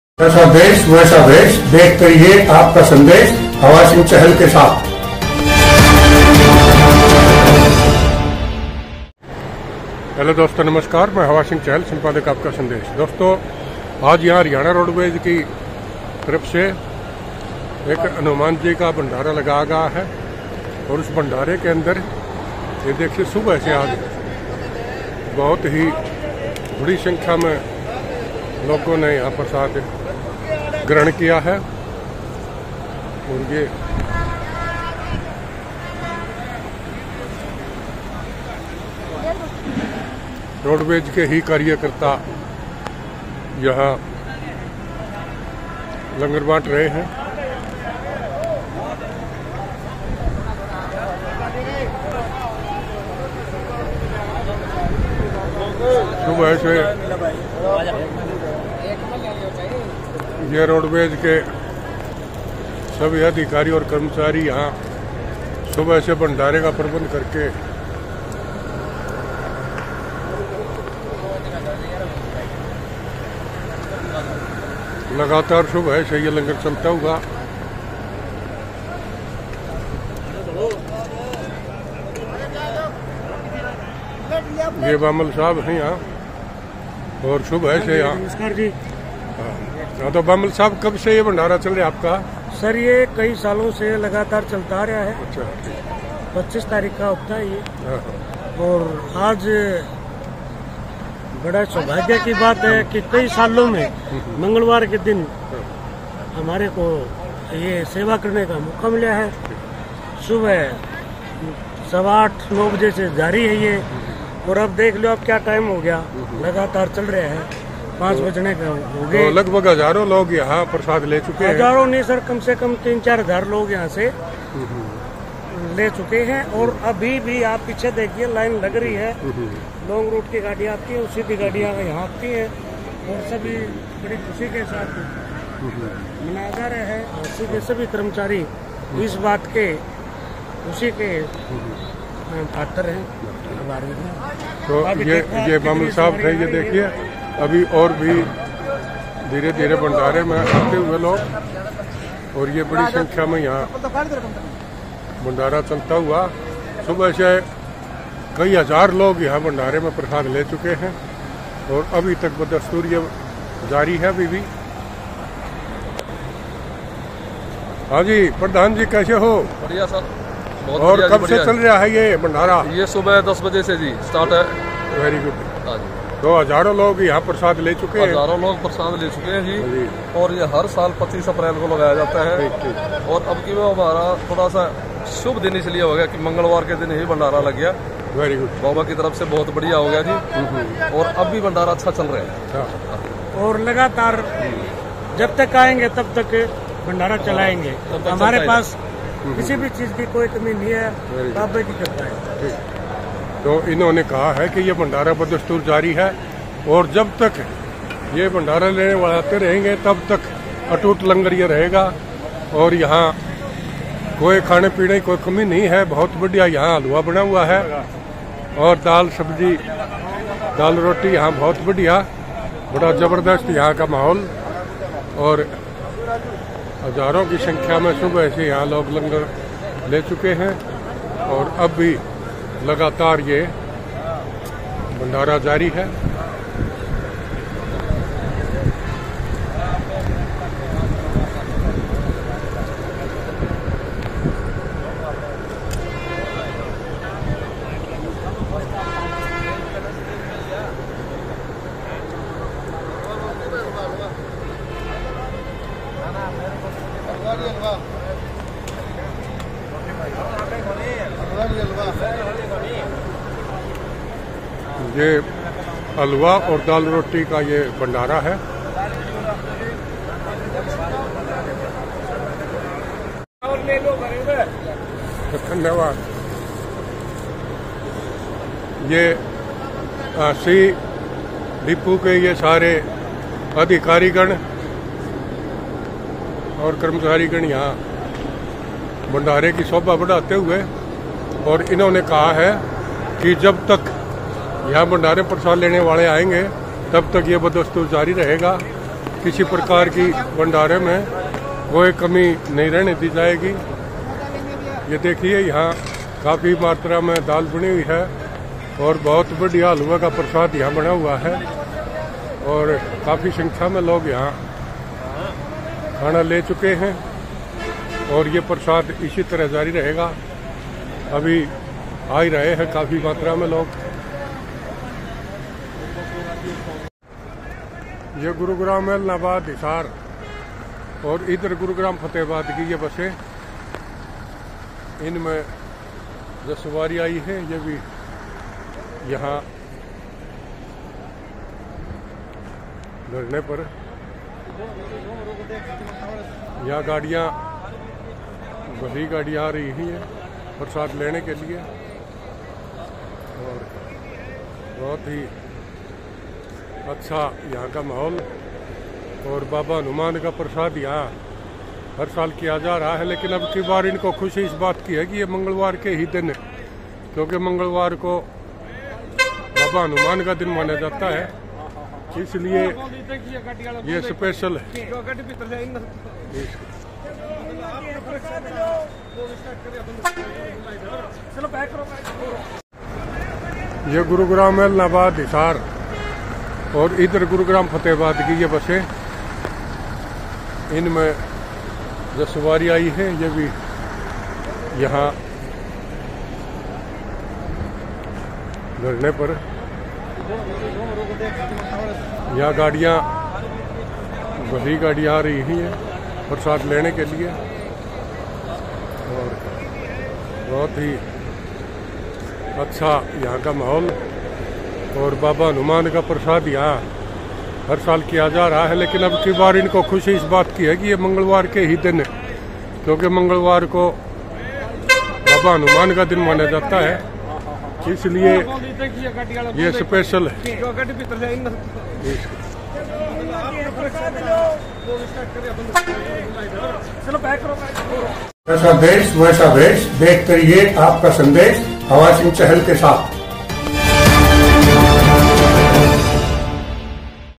हाँसी के रोडवेज स्टाफ़ ने लगाया भंडारा, हज़ारों लोगों ने ग्रहण किया प्रसाद!
हाँसी डिपो के अधिकारी व कर्मचारी हर साल लगाते हैं भंडारा!